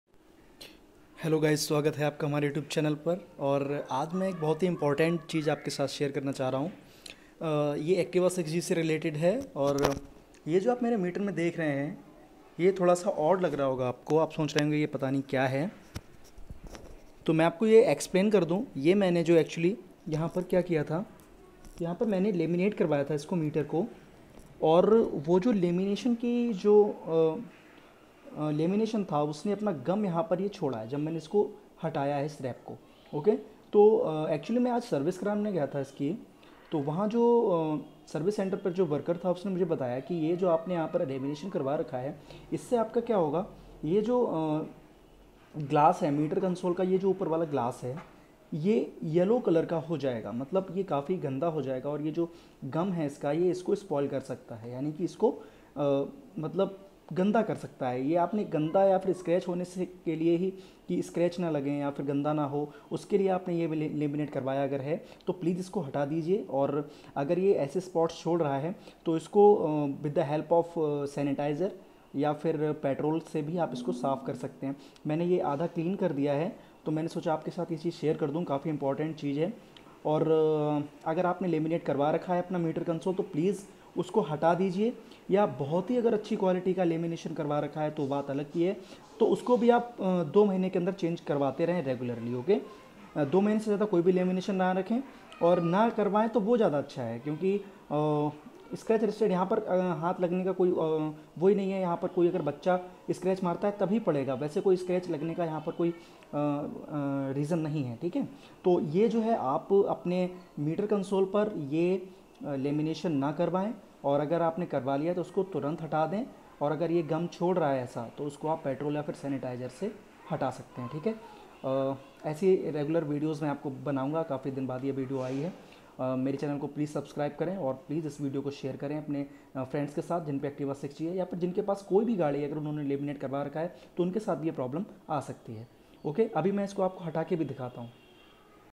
Avoid Lamination on Metre consol glass. Activa 6G . Very important please watch.
Information about Activa 6g mtre cinsol glass .